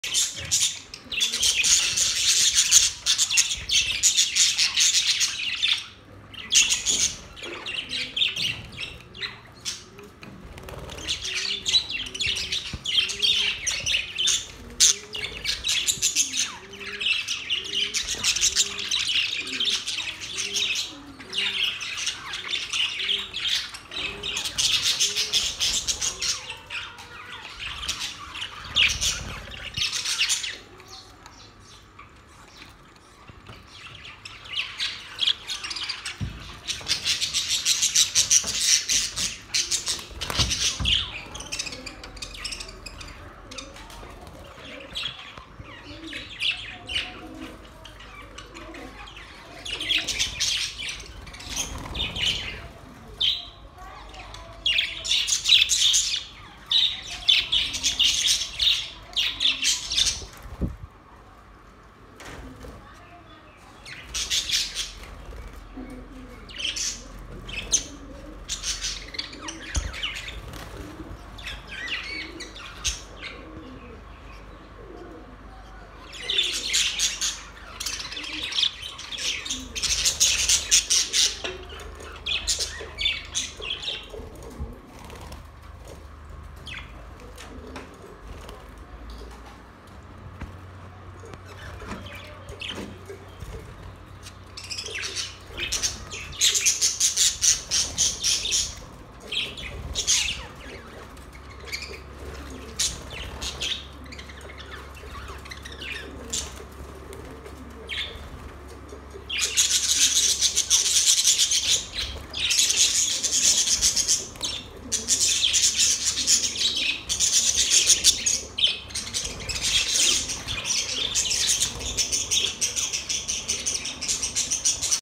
My Budgies very Happy | singing happy sounds | My Happy Budgies fly in cage
It sounds like your budgies are having a great time singing and flying in their cage! Happy budgies are a joy to have as pets